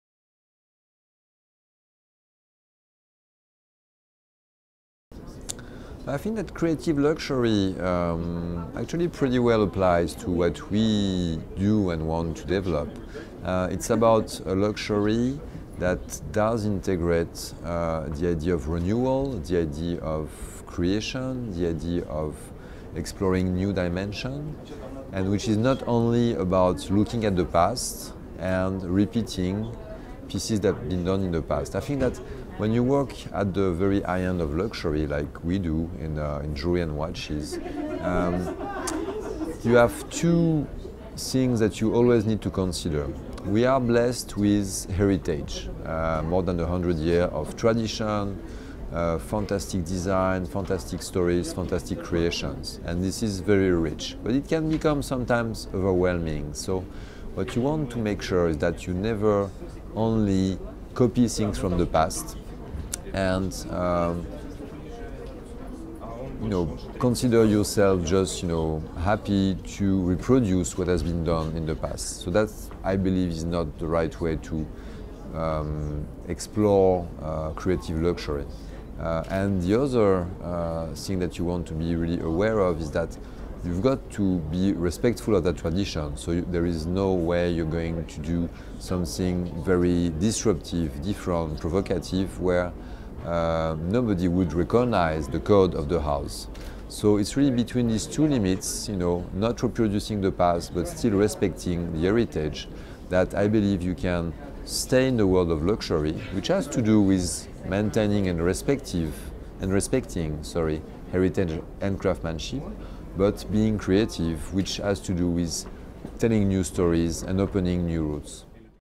LJ Plus Issue 09: Creative Luxury - Nicolas Bos
Interview with Van Cleef & Arpels Vice President & Creative Director, Nicolas Bos

《Art of Timeless Beauty 愛上美的藝術 戀上美的傳承》

一段浪漫的愛情故事，成就一個經典頂級珠寶及鐘錶品牌的傳奇。Van Cleef & Arpels梵克雅寶的故事源自百多年前的一段良緣。來自珠寶家族的Estelle Arpels與Alfred Van Cleef共諧連理，這段矌世姻緣奠定了一個傳奇的誕生。梵克雅寶所設計生產的珠寶首飾，都承載著一個由愛而起的緣分，難怪一百年來梵克雅寶一直予人充滿詩意浪漫的形象感覺。 Desire：以Landscape呈...現珠寶設計之美 / Bespoke：專訪創意總監Nicolas Bos / Edutainment：專訪梵克雅寶珠寶學院院長Marie Vallanet-Delhom